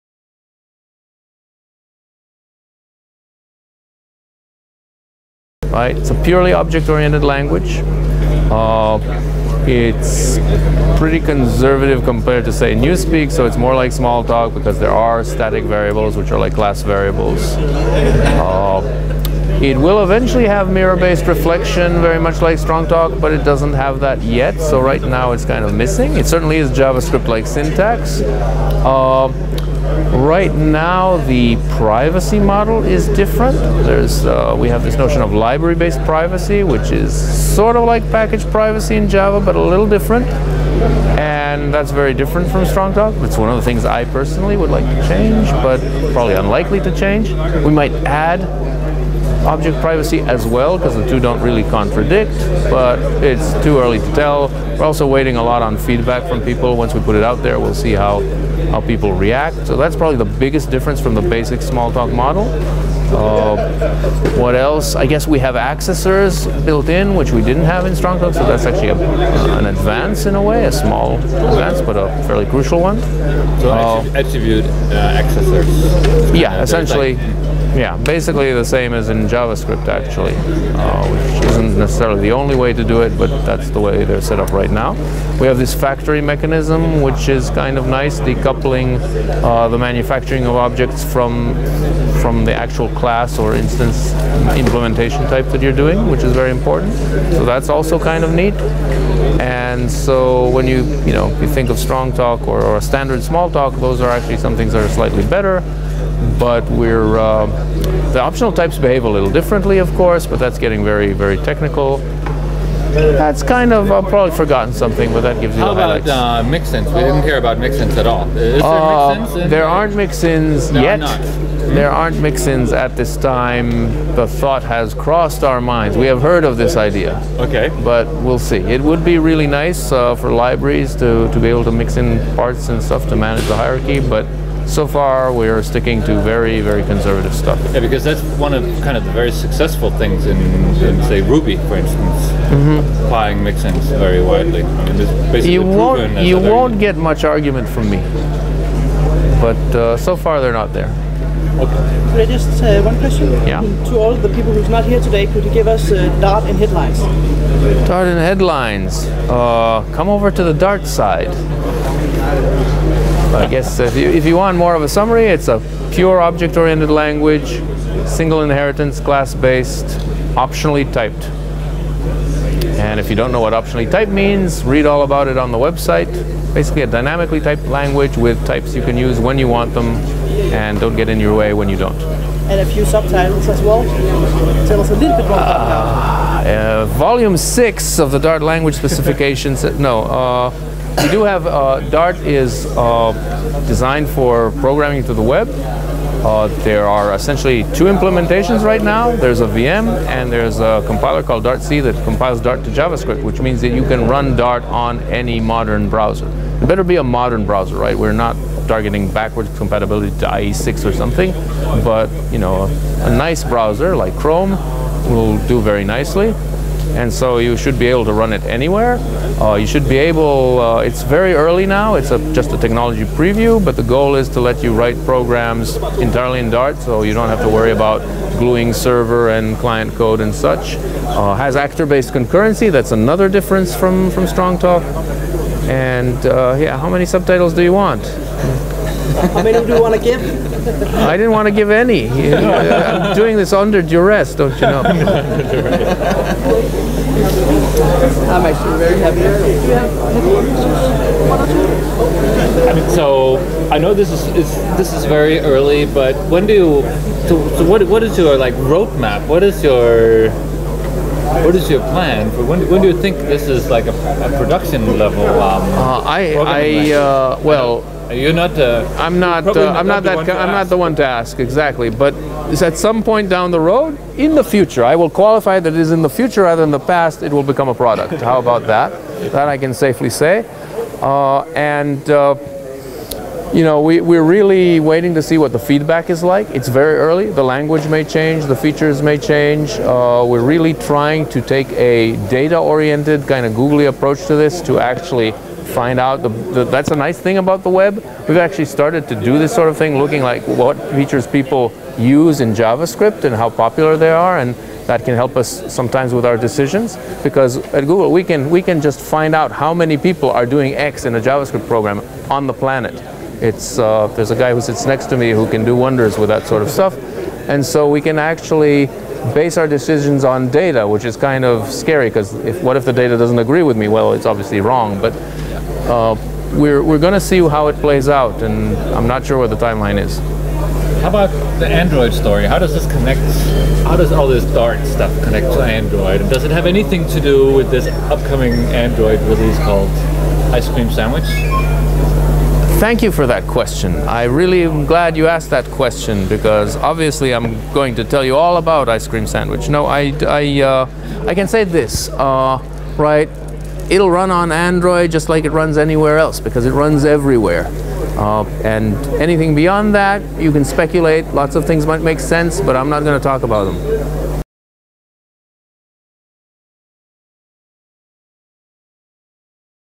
Interview with Gilad Bracha about Google Dart • GOTO 2011
This presentation was recorded at GOTO Aarhus 2011

Gilad Bracha - Software Engineer, Google Inc.

ABSTRACT
Interview from the World Premiere of Google Dart at GOTO Aarhus 2011.